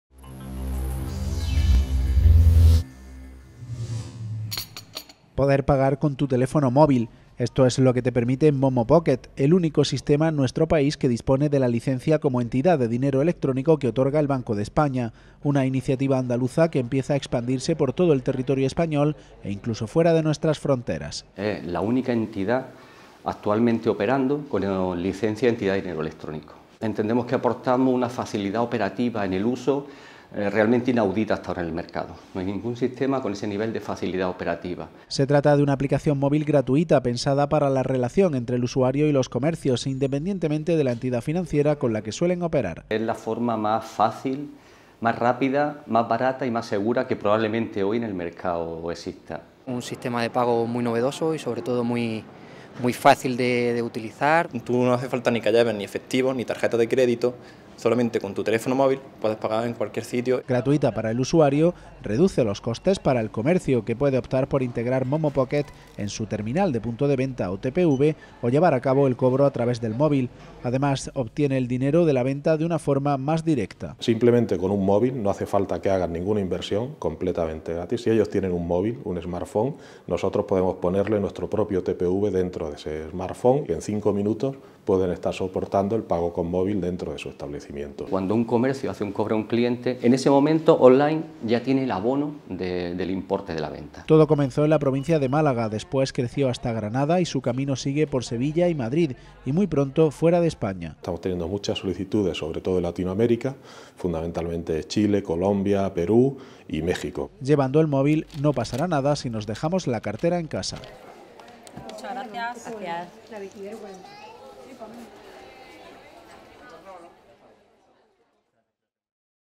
HDL Momo Pocket, primer sistema de pago en el móvil con proyección internacional desde Andalucía
El pago con móvil comienza a ser una realidad en nuestro país. La aplicación Momo Pocket es el primer sistema que cuenta con la licencia como entidad de dinero electrónico que otorga el Banco de España. Un proyecto andaluz que se extiende ya por todo el territorio nacional y que ha generado interés en países como Chile, Colombia, Perú y México.